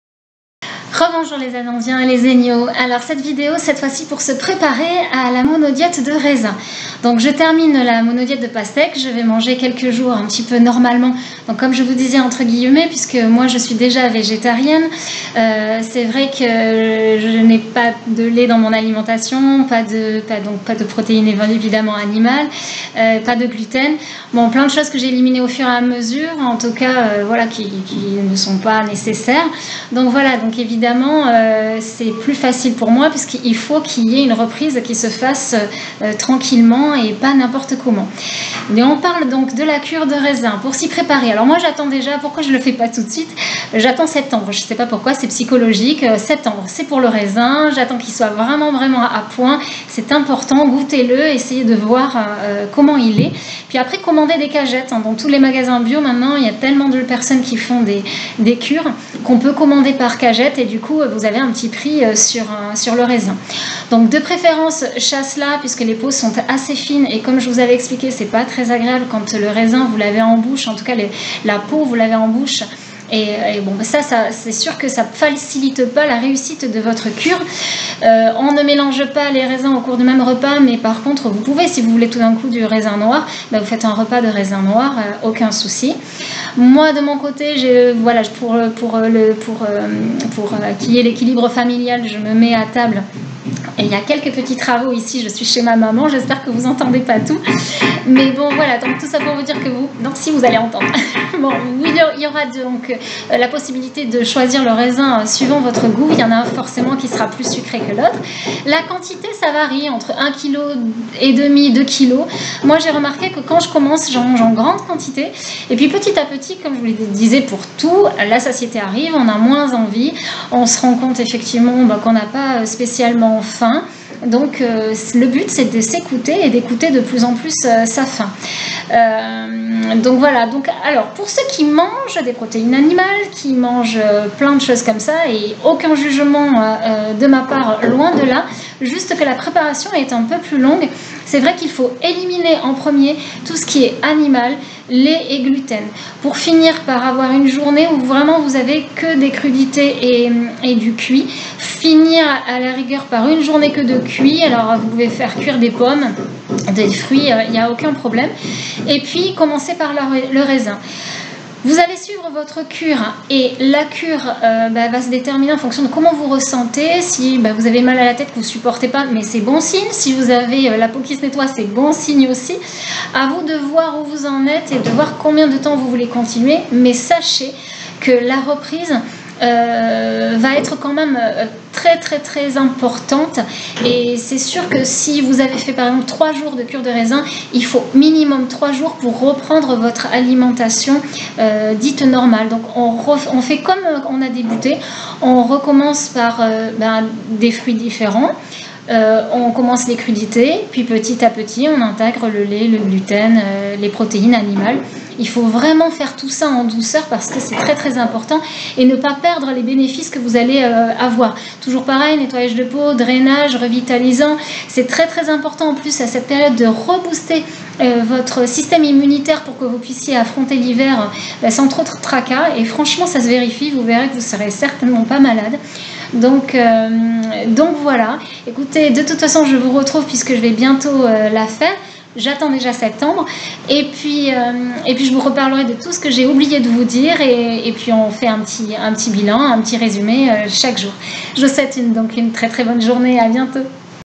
Préparation à la mono diète de Raisin ANANDA
De votre alimentation dépend votre santé et il est bon de vous informer convenablement .
Prenez vos précautions,faites vous suivre et conseillé par des professionnels de la santé.
Nous sommes tous différents et ce qui est bon pour l'un ne l'est pas forcement pour l'autre . Aussi faites vos expériences en accord avec vous même et sans jugements.
Soyez curieux !